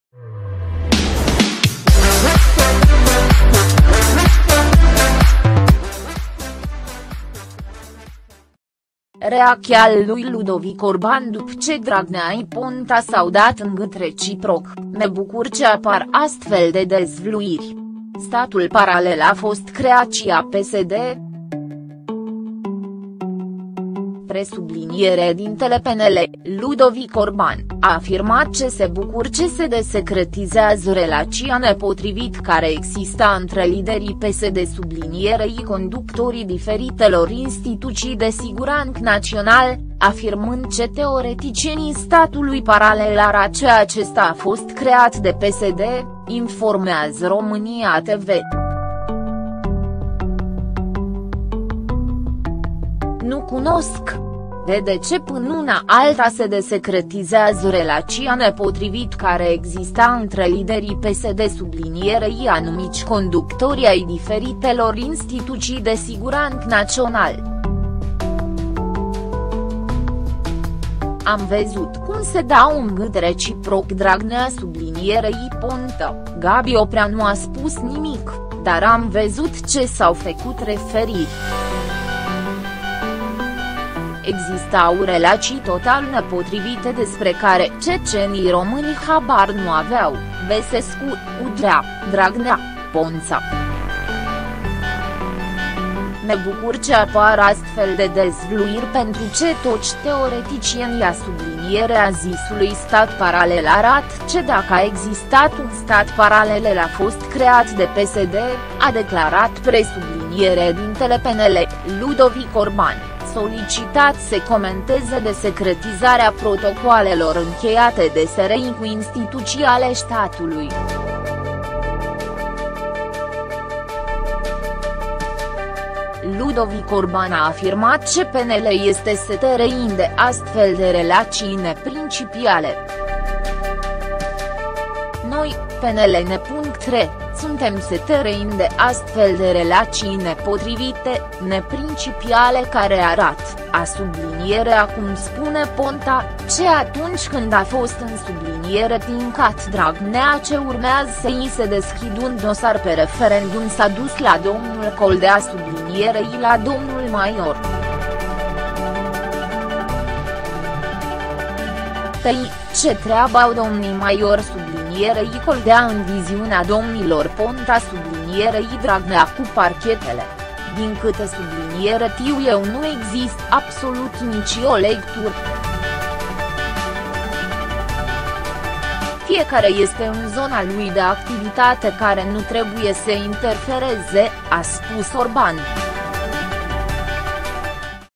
Reacția lui Ludovic Orban după ce Dragnea și Ponta 's-au dat în gât reciproc
Reacția lui Ludovic Orban după ce Dragnea și Ponta 's-au dat în gât reciproc': 'Mă bucur că apar astfel de dezvăluiri. Statul paralel a fost creaţia PSD'